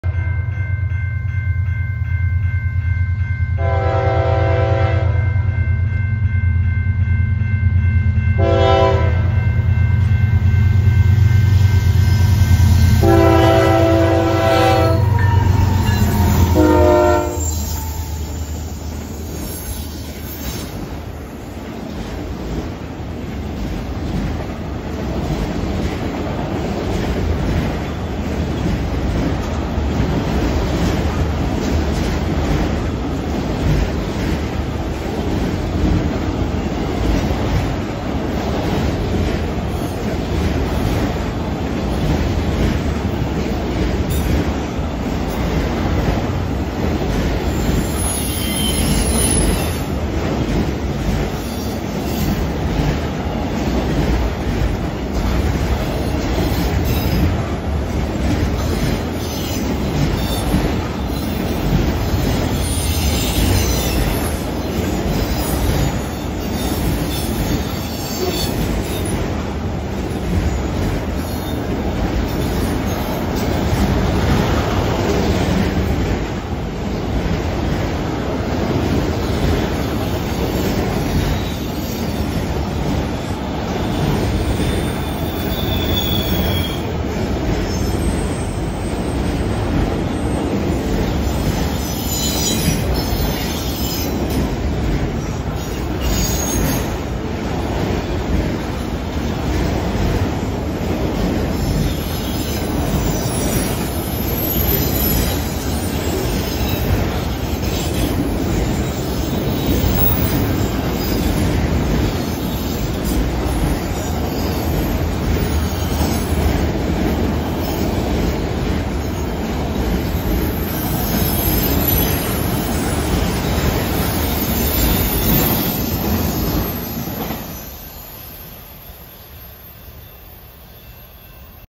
7/11/24 CN Train L 543
7/11/24 20:13 78 degrees at CN MP 202 Charlotte as CN 2319 and CN 8023 power Train L 543 across Lawrence Ave./ M -79 and into the curve. Thanks for the 👋 ! 😀 Full 2:12 video.